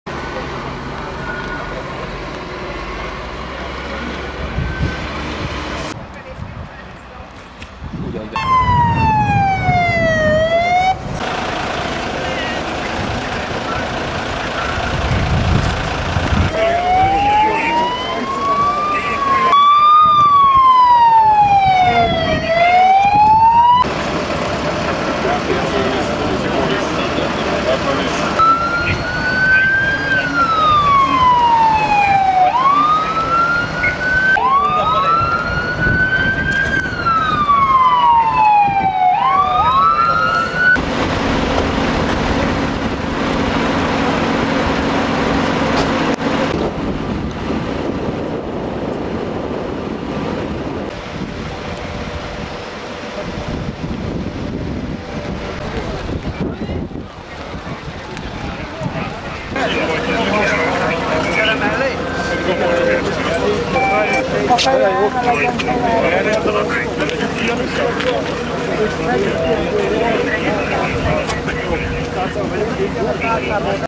Rabszállító tömeges karamboljának mentési gyakorlata az M4-es autópályán
A feltételezés szerint 2019. május 9-én egy gyerekeket szállító busz, egy rabszállító és két személygépkocsi ütközött össze Hajdú-Bihar megyében, az M4-es autópálya 212-es kilométerénél. A felvételt Papp-Kunkli Nóra (Hajdú-Bihar Megyei Katasztrófavédelmi Igazgatóság) készítette.